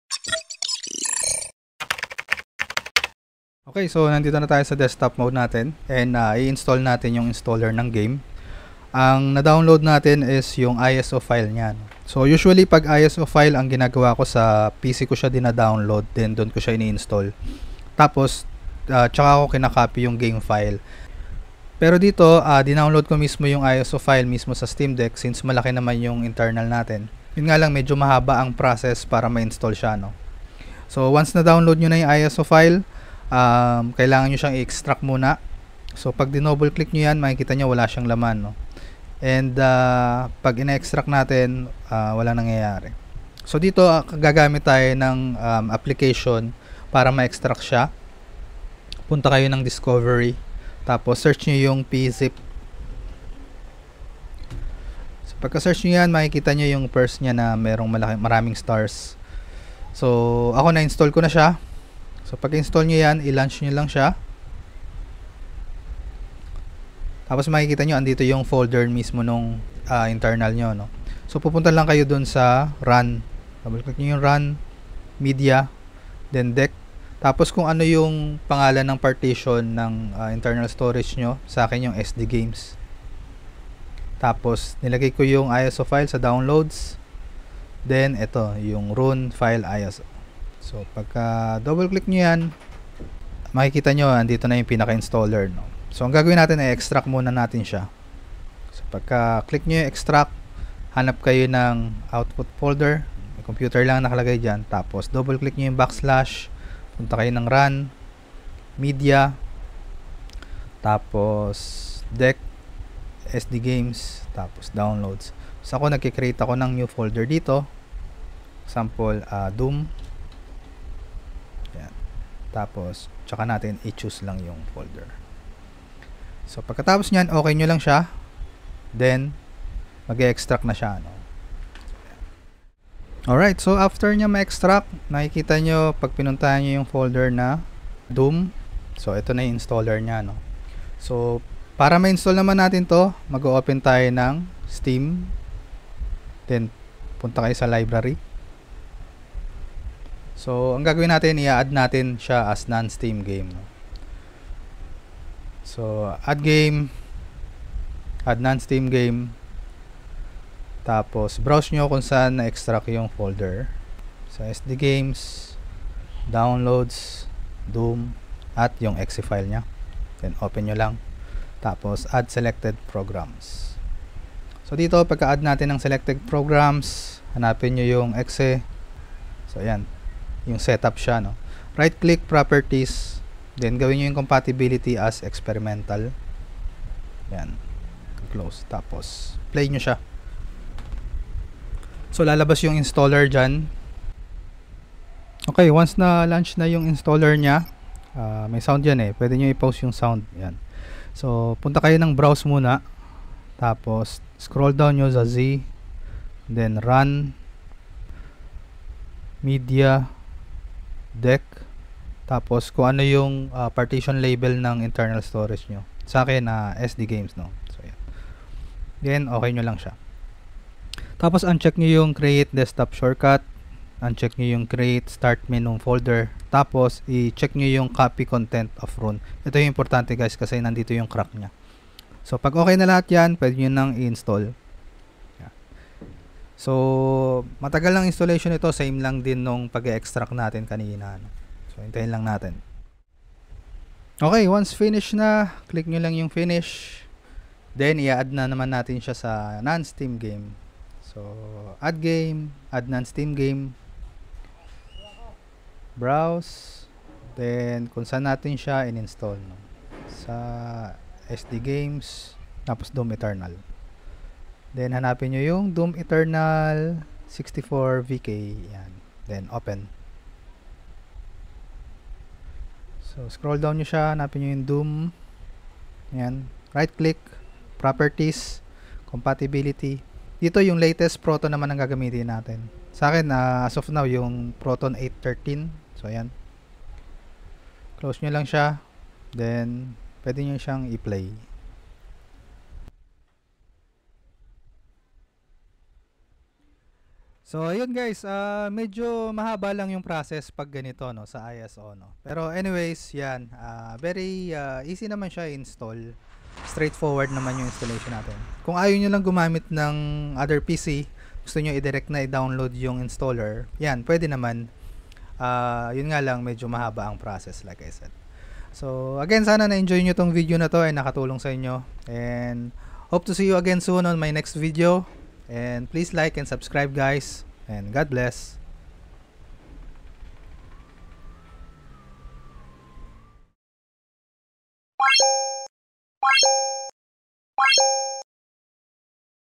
How to Install .ISO on Steam Deck | Filipino Tutorial Guide with Eng Sub
This is just a reuploaded video for quick guide purposes. I hope it helps